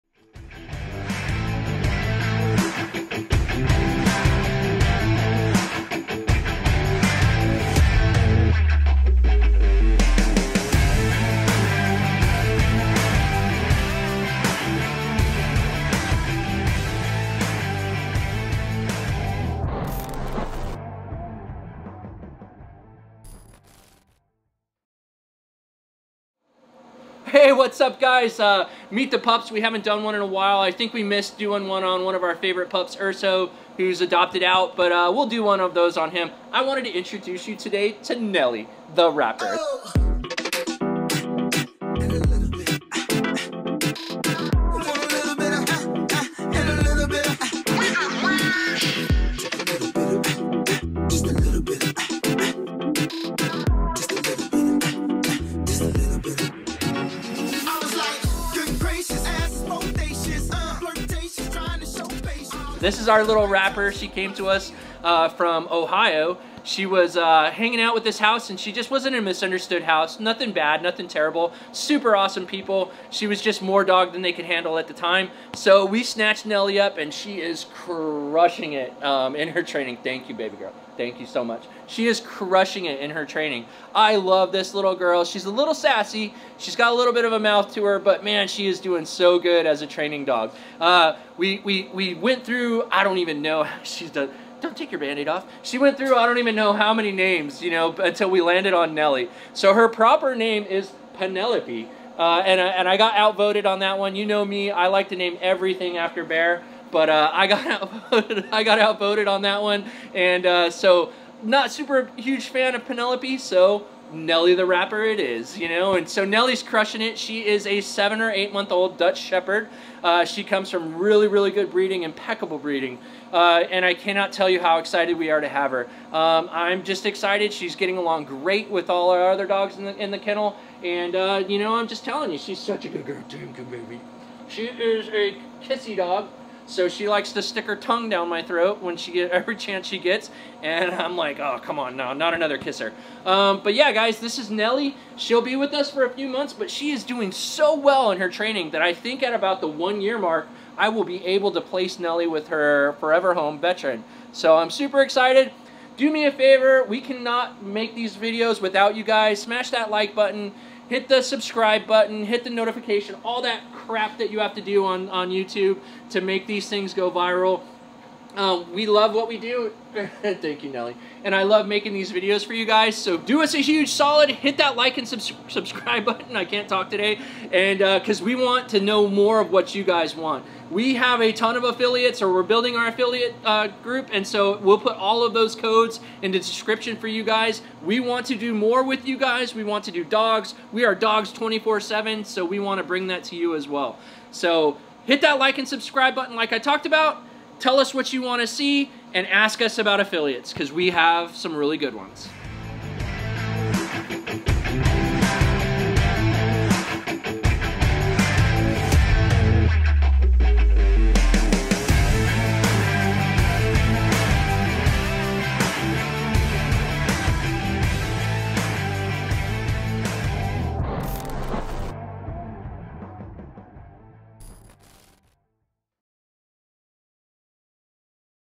Meet the rescue NELLY (Penelope)
Nelly was a good girl just a little misunderstood. She is a feisty thing and had some jealousy issues over the baby in the house. That was her momma and she felt like she was the baby. She loves so hard that it wasnt the right fit. She was in an amazing loving home (which is new for our Rescues). She has gone on several trips with me and is well on her way to becoming a service dog.